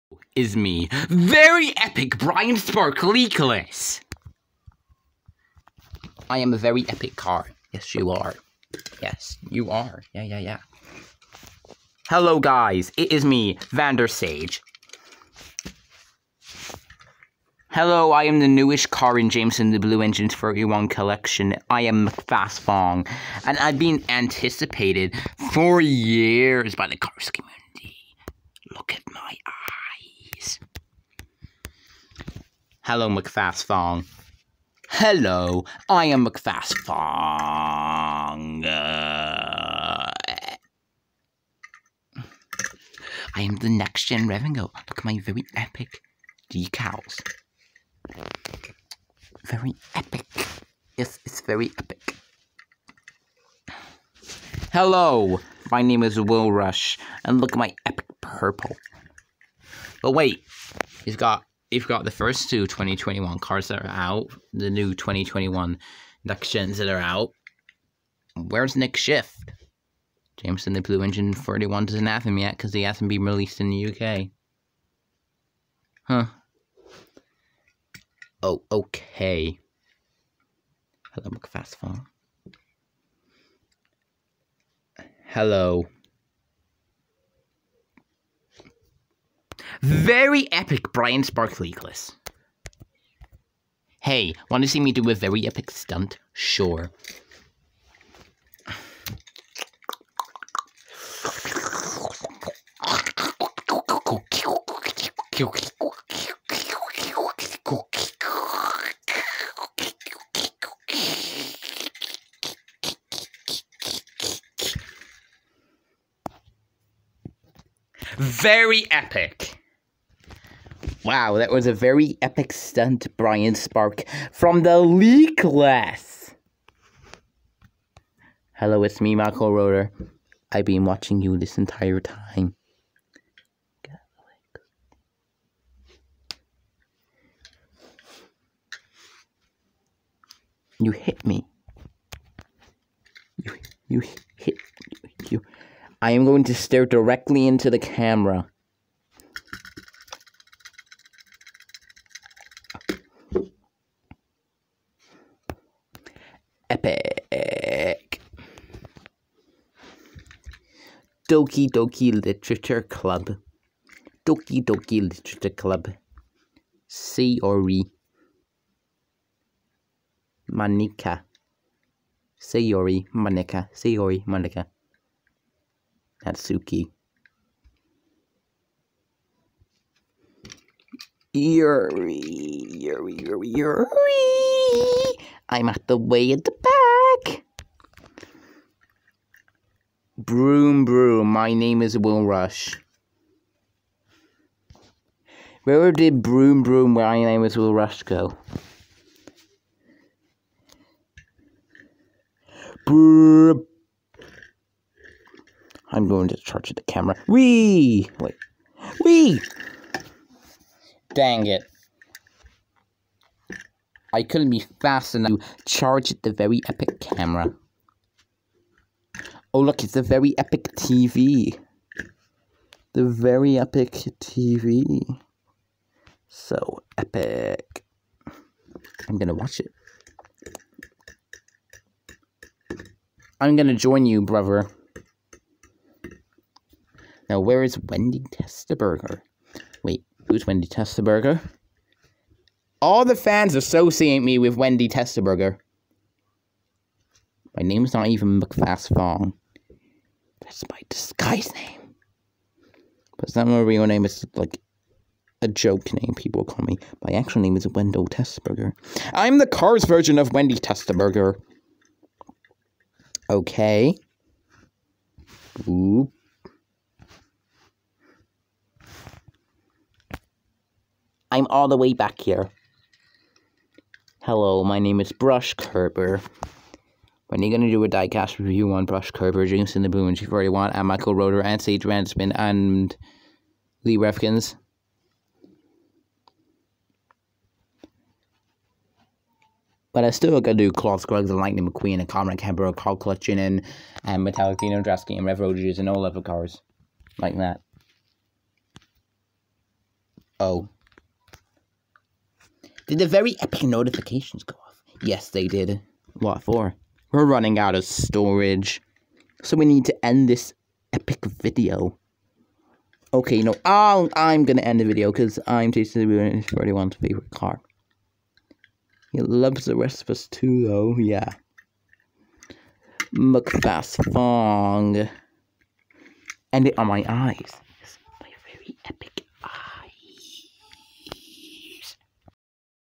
M Fast Fong and others yell at a camera
Cars yell at the camera for 5 minutes or something...
LEAK LESS BRIAN SPARK